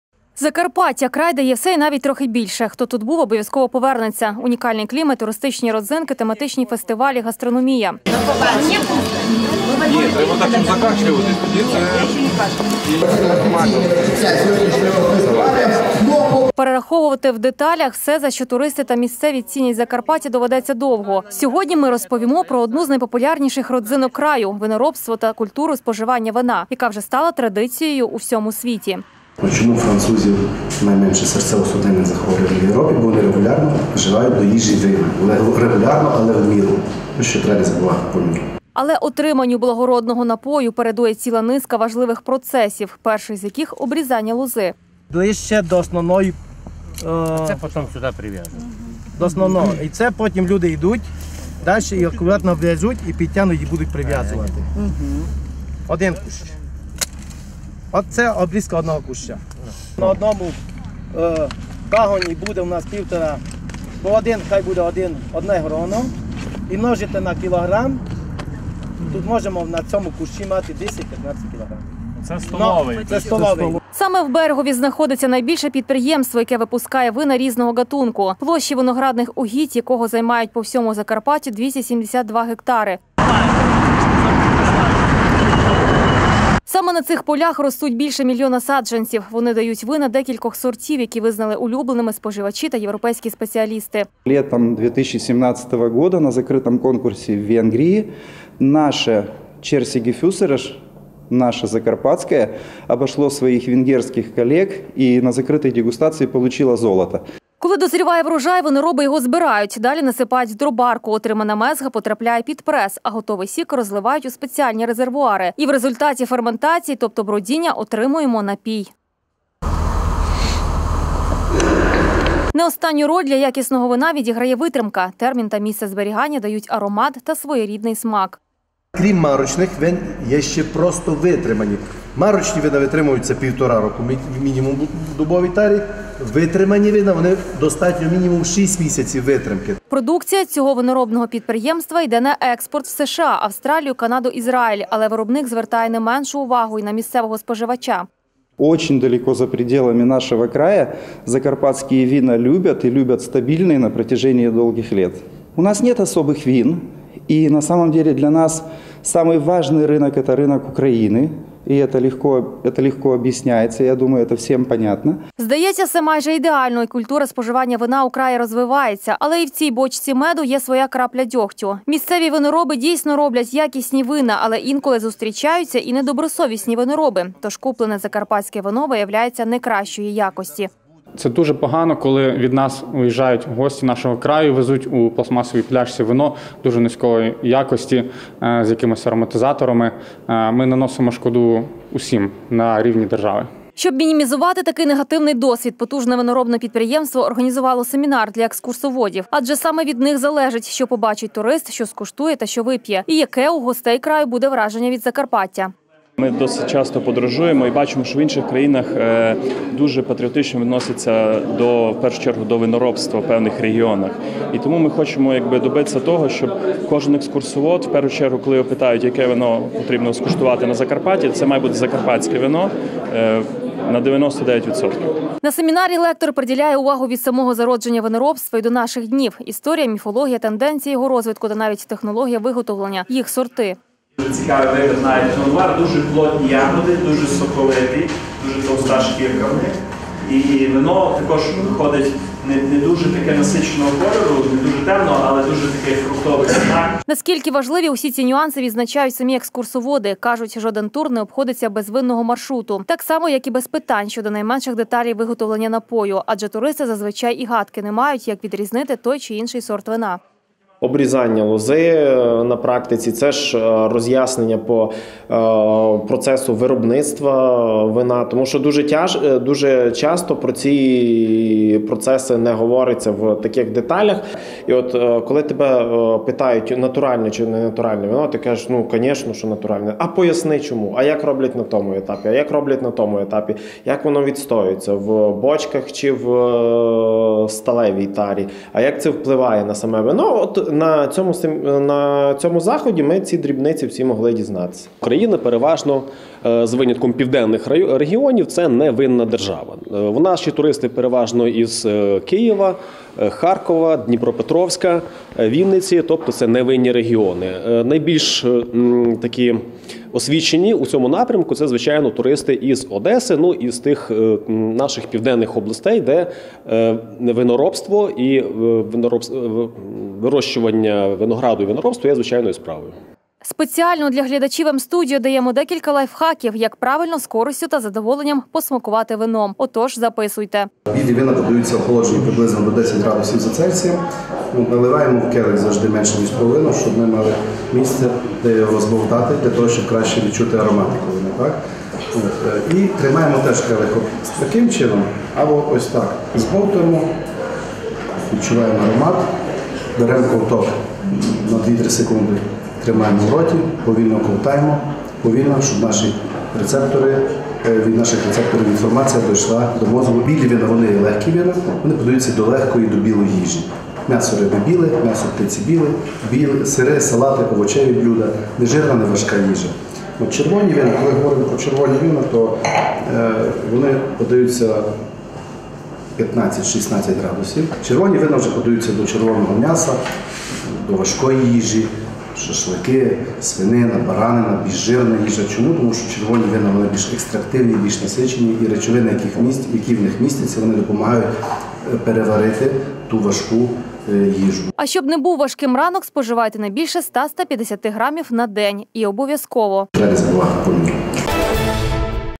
Закарпаття - край вина
Пріоритетна галузь для  Закарпаття  - розвиток виноробства. Журналісти «М-студіо» побували на одному з найбільших виноробних підприємств, та дізнались про те, що допомагає, чи навпаки, заважає Закарпаттю отримати статус  краю виноробів, про культуру споживання вина та розвиток туристичного напрямку в цій сфері.